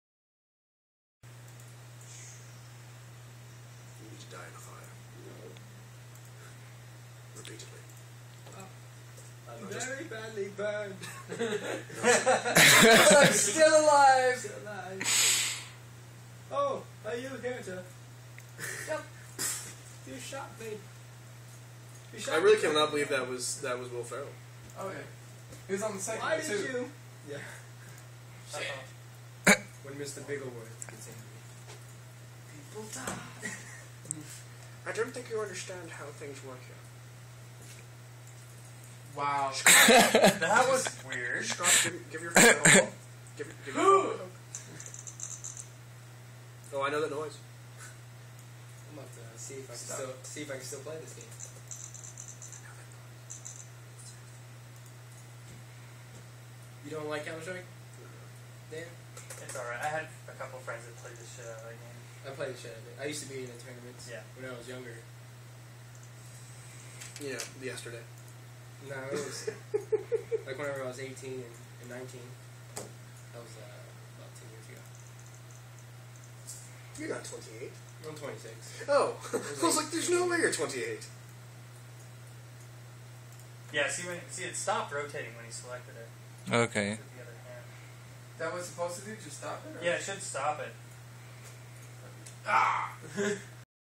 Telekinect Meeting 1: Clip 1
08/24/11 - Working on our senior project, Telekinect, which is being made with Unity3D and the Kinect